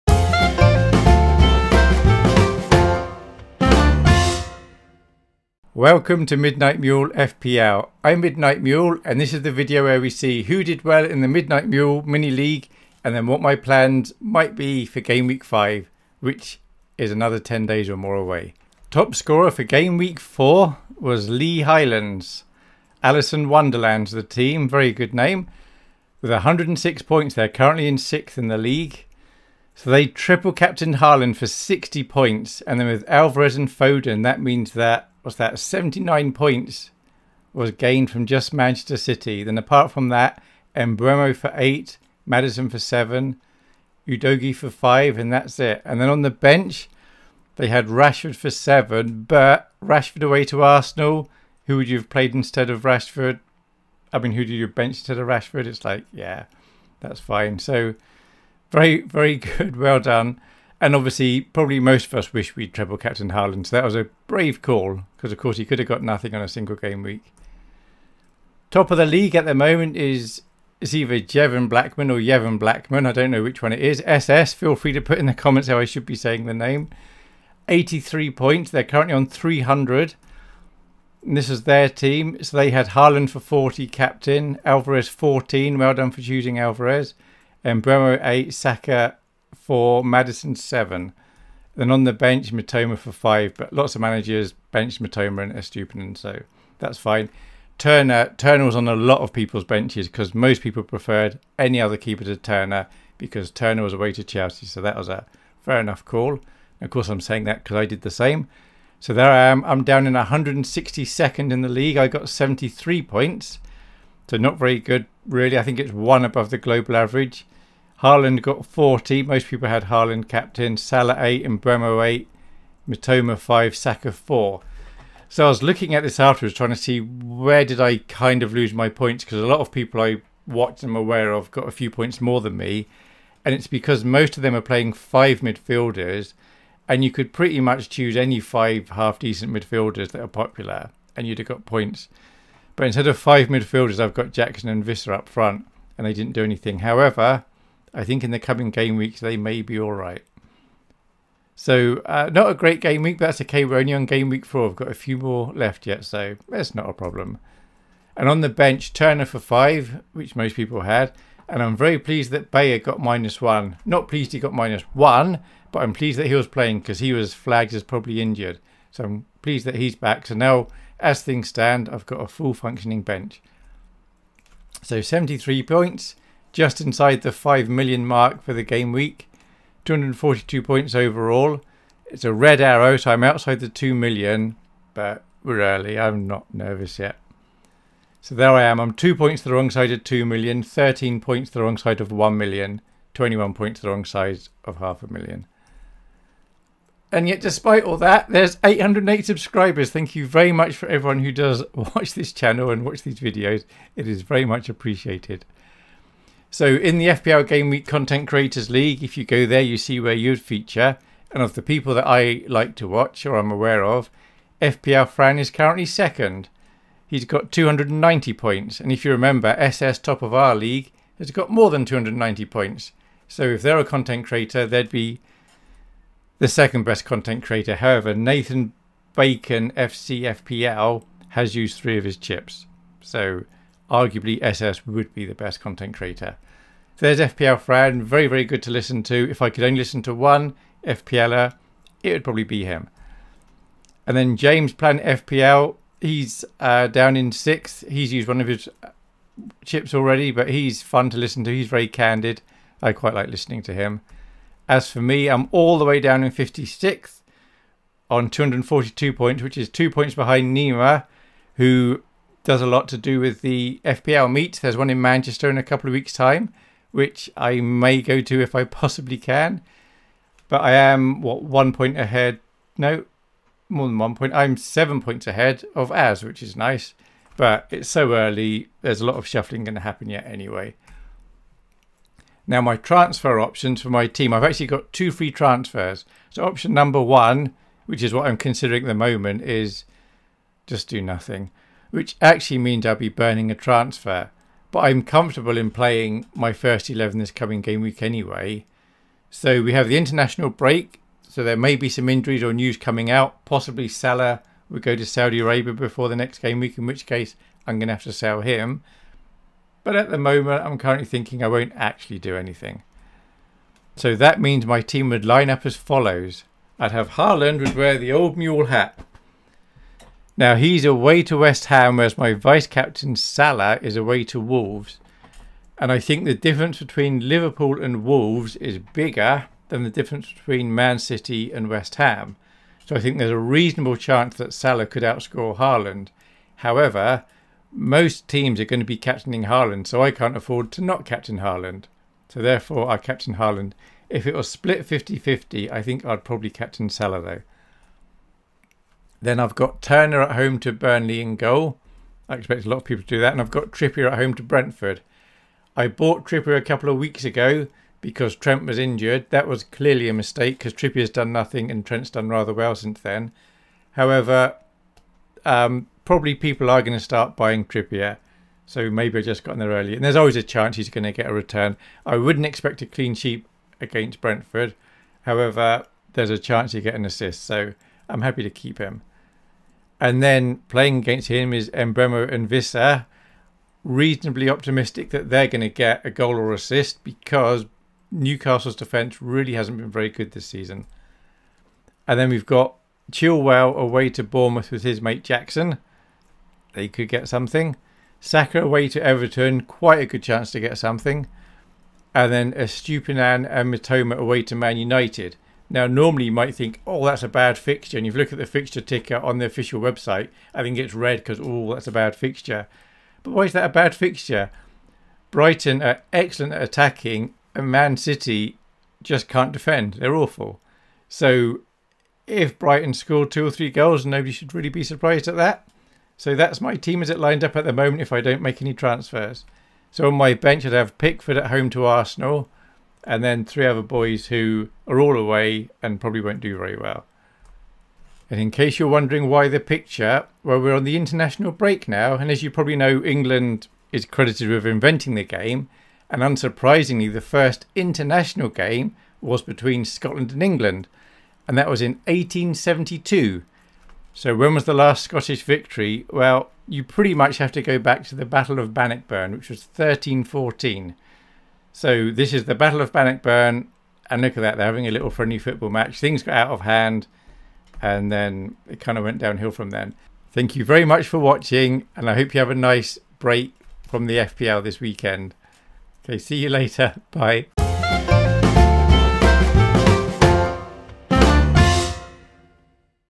My Gameweek 05 FPL Team Reveal
In this video I look at the Midnight Mule League and the Content Creators League (which anyone can see themselves in). I then look at my transfers and team for Gameweek 05 and my captaincy choices..

Thanks,

Midnight Mule